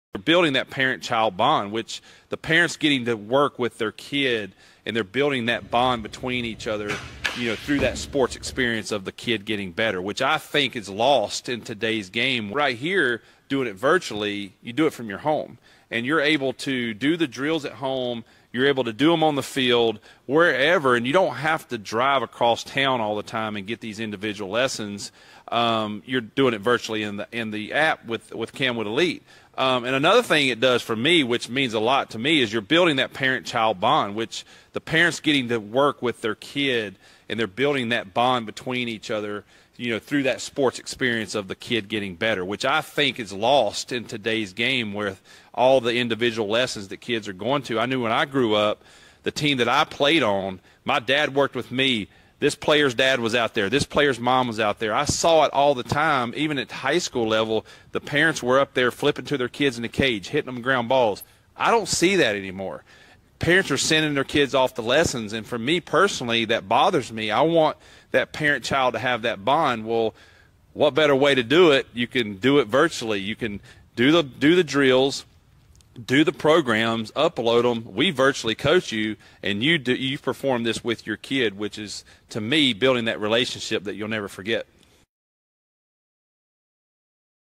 Start training together!! Some of the best moments as a young athlete is going out to the back
Start training together!!

Some of the best moments as a young athlete is going out to the back yard, or field with mom or dad and working on your game together.

A lot of that these days has been lost driving across town, or sending your kids to an instructor…well that’s one of the reasons we designed the
Camwood Elite.

Not only to save you money on private lessons, but to have everything in one place. Demonstrations videos, day by day workout plan, and Professional coaches to help you with any questions along the way. So you and your kid can do it at home together! 💯

Now go get signed up today. It will be one of the best choices you make for you and your kid! 📲